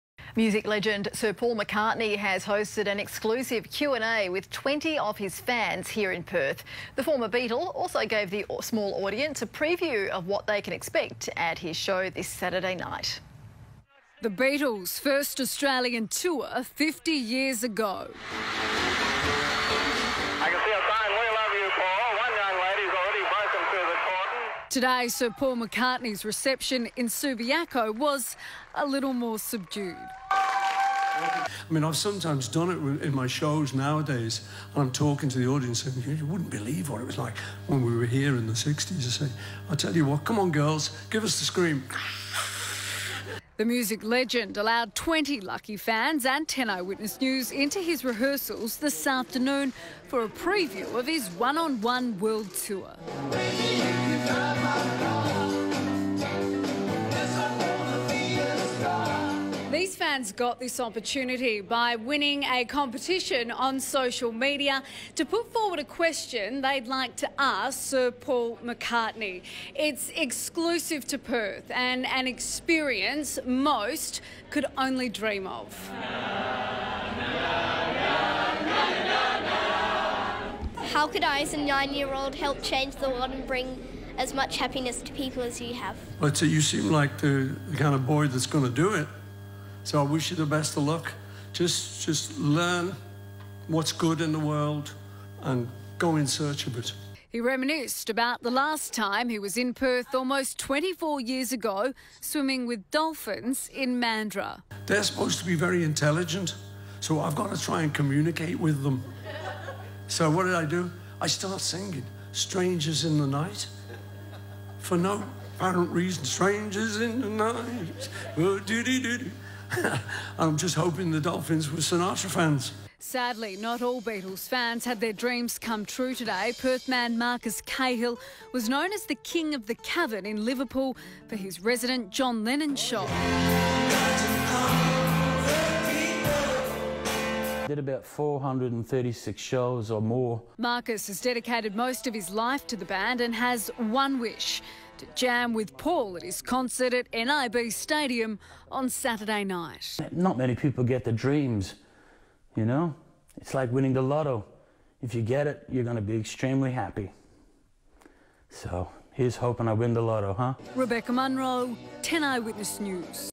Marcus Cahill to Sir Paul McCartney wannajam ?
Here I am trying to get the opportunity to jam on stage with my idol SIR PAUL McCARTNEY